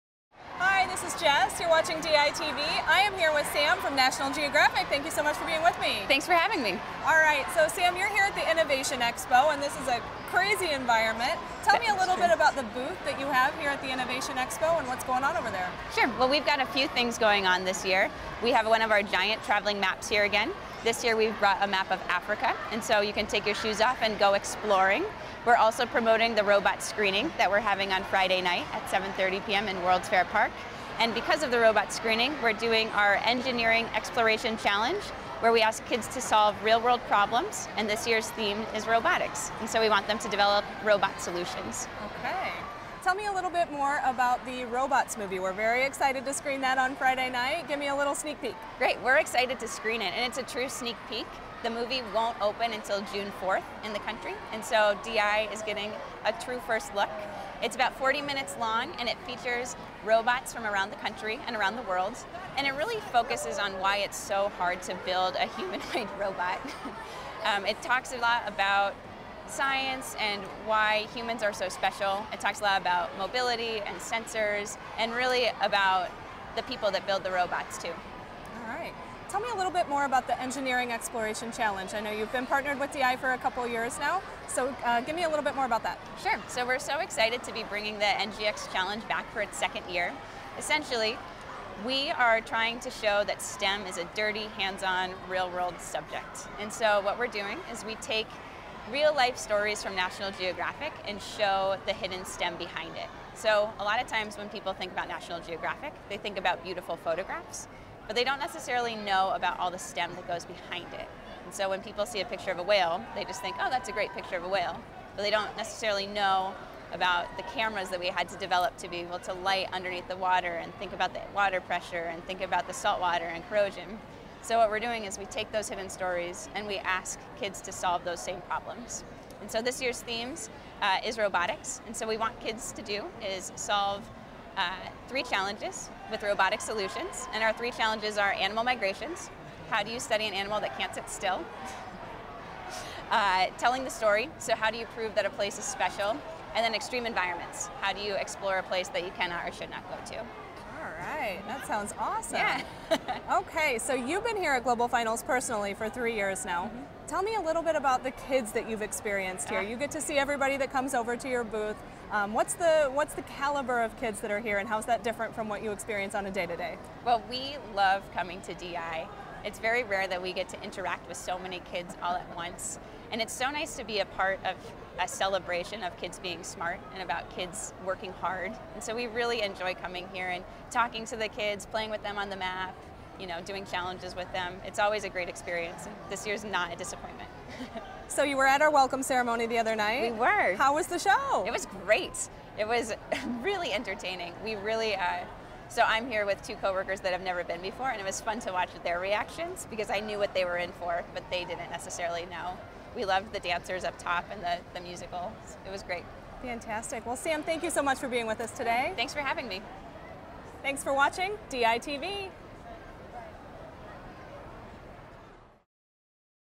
National Geographic Project Manager Samantha Zuhlke
Samantha Zuhlke, Project Manager at the National Geographic Society, discusses the Nat Geo booth at the Innovation Expo, the premiere of National Geographic's "Robots" at Global Finals Movies in the Park and the Engineering Exploration Challenge, which encourages students to create robotic solutions.